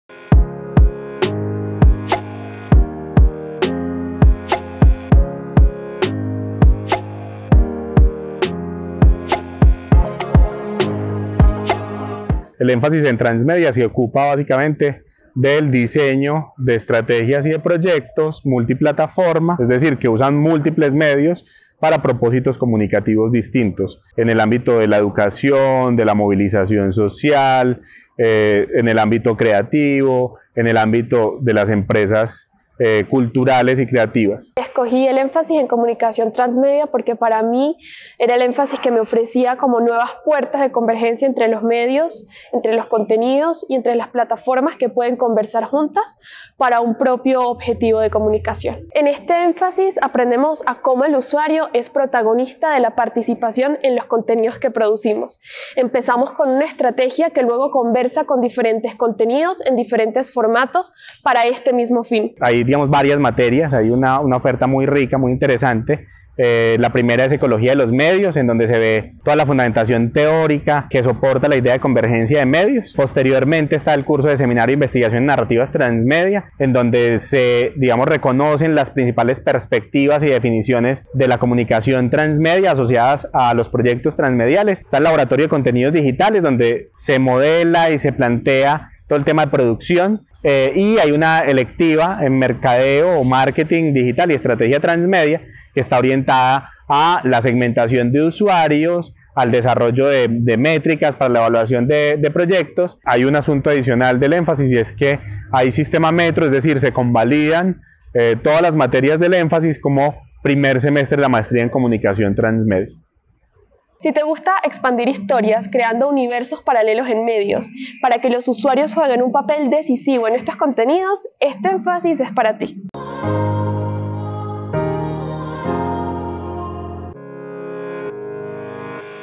Énfasis en Comunicación Transmedia
Conoce más sobre el énfasis en Comunicación Transmedia, que hace parte de la oferta de énfasis del programa en Comunicación Social de la Escuela de Artes y Humanidades de EAFIT.